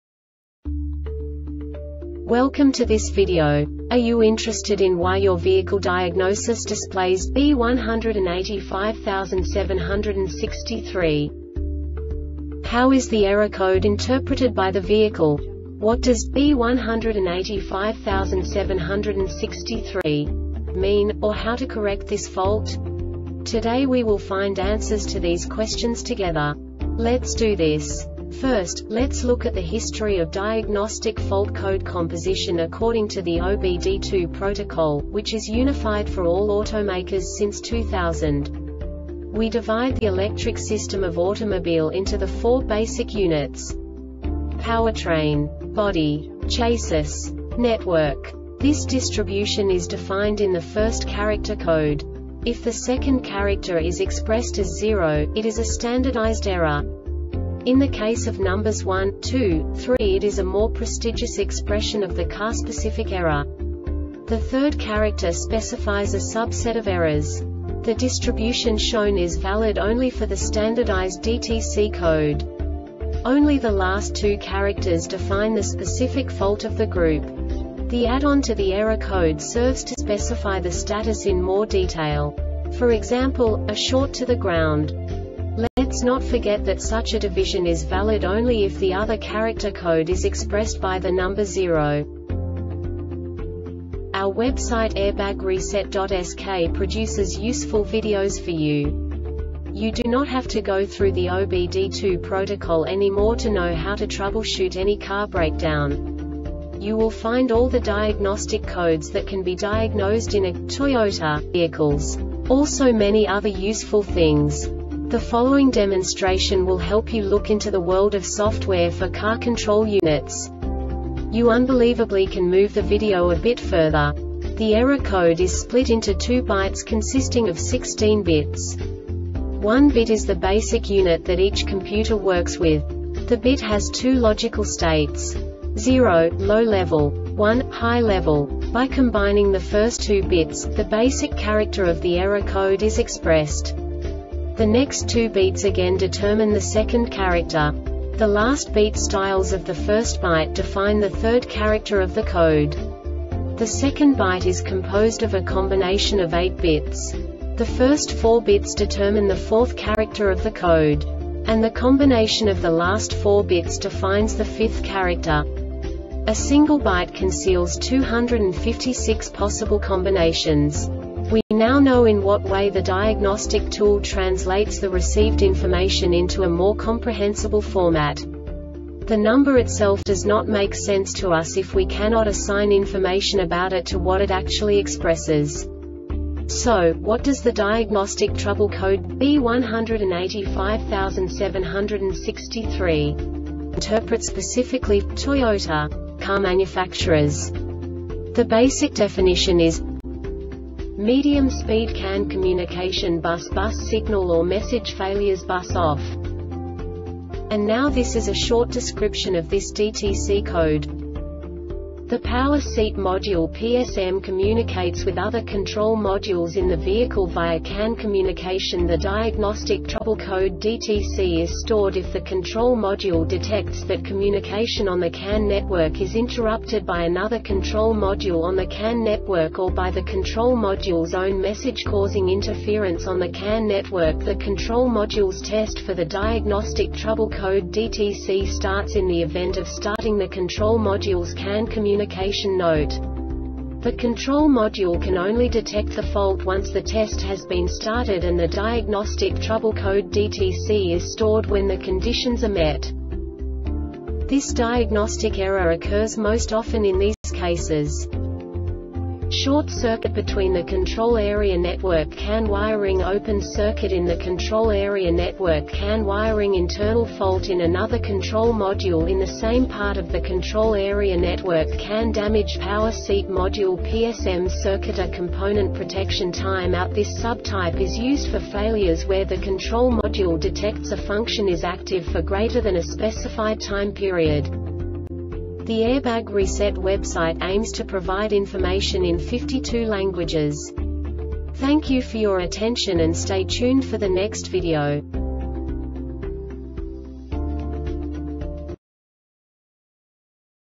DTC Toyota B1857-63 Short Explanation
Contents:
0:21 Basic DTC analysis according to OBD2 protocol standard.
1:48 Insight into programming
2:58 A diagnostically understandable description of the Diagnostic Trouble Code
3:05 Basic error definition

Make:
Toyota
Code:
B1857 63
Definition:
Medium Speed CAN Communication Bus. Bus Signal or Message Failures. Bus Off
Description:
The Power seat module (PSM) communicates with other control modules in the vehicle via CAN communication.The diagnostic trouble code (DTC) is stored if the control module detects that: Communication on the CAN network is interrupted by another control module on the CAN network or by the control modules own message causing interference on the CAN network.The control modules test for the diagnostic trouble code (DTC) starts in the event of: Starting the control modules CAN communication.Note! The control module can only detect the fault once the test has been started and the diagnostic trouble code (DTC) is stored when the conditions are met.
Cause:
Short-circuit between the control area network (CAN) wiring.Open-circuit in the control area network (CAN) wiring.Internal fault in another control module in the same part of the control area network (CAN).Damaged power seat module (PSM).
Failure Type:
CircuitorComponent Protection Time-Out
This sub type is used for failures where the control module detects a function is active for greater than a specified time period.